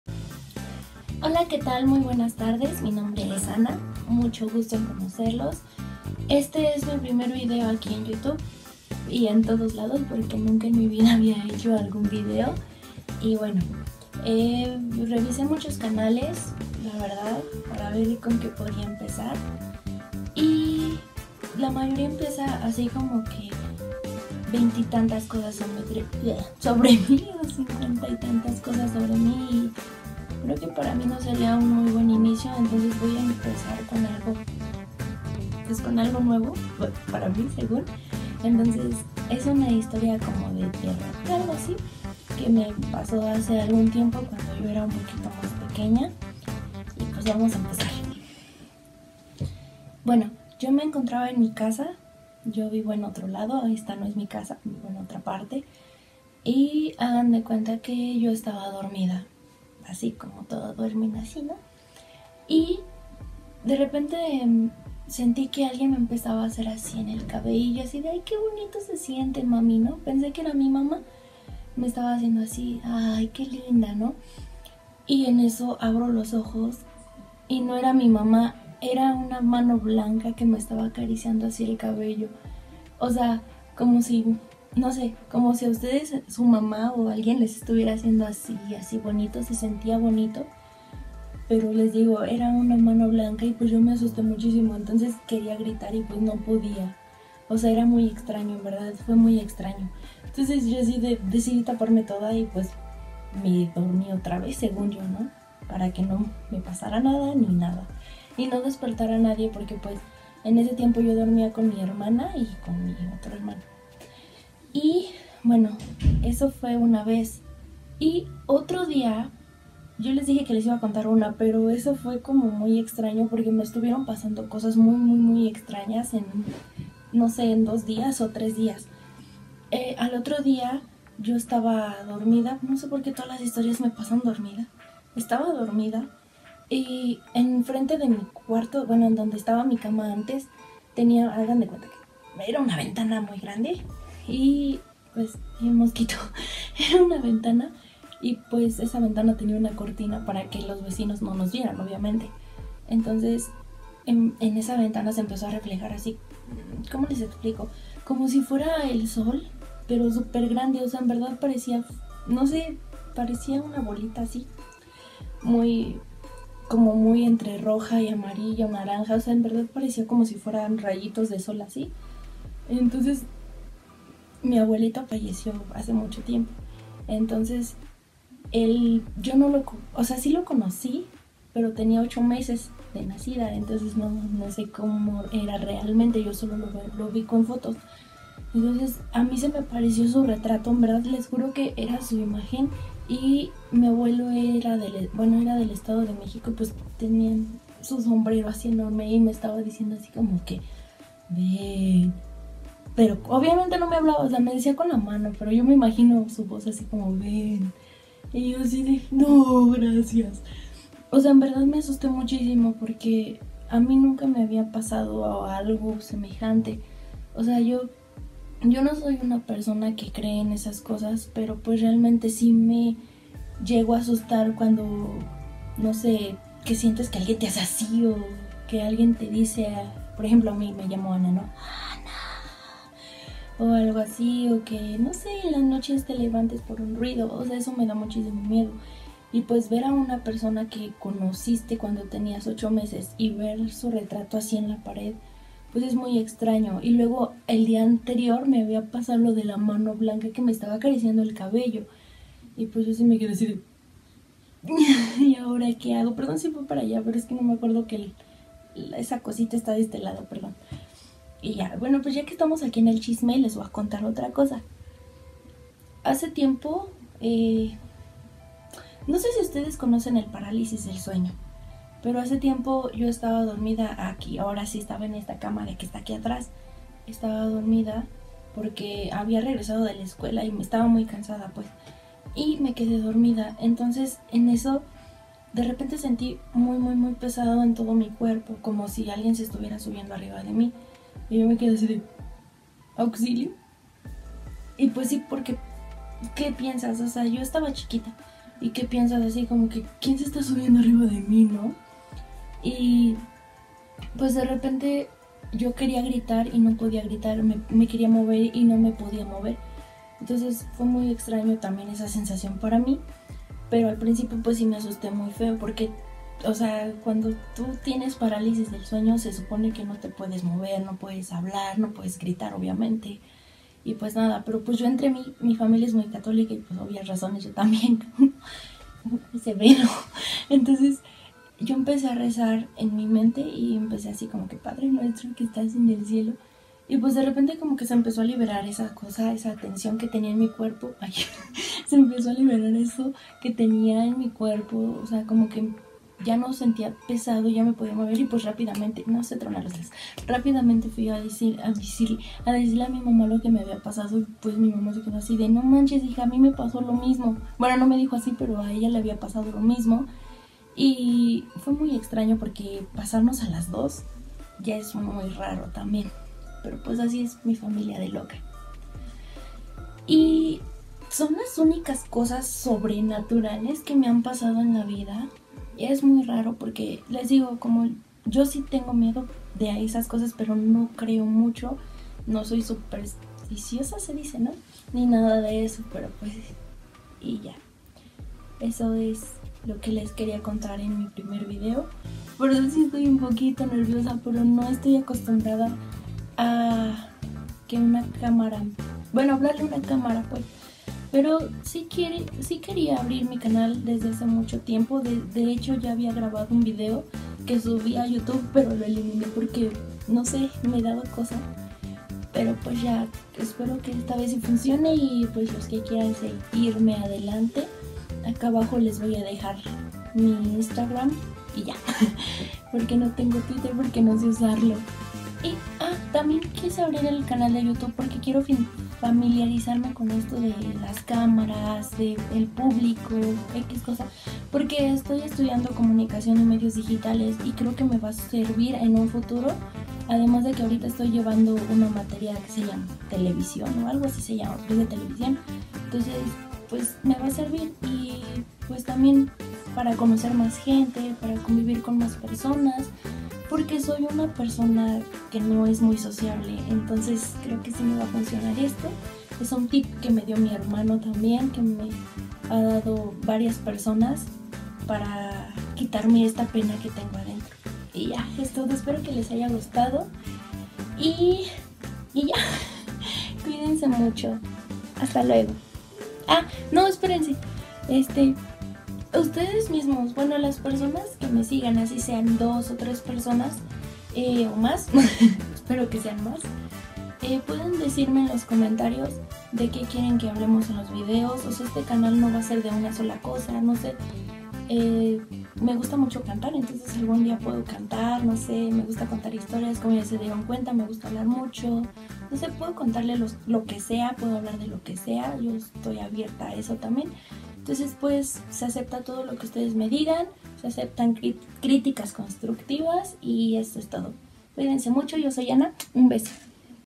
Mi Primer Video
Hola a todos, este es mi primer vídeo, en el cual les cuento 3 historias sobrenaturales que me han pasado a lo largo de mi vida, espero les guste.
Un beso ♥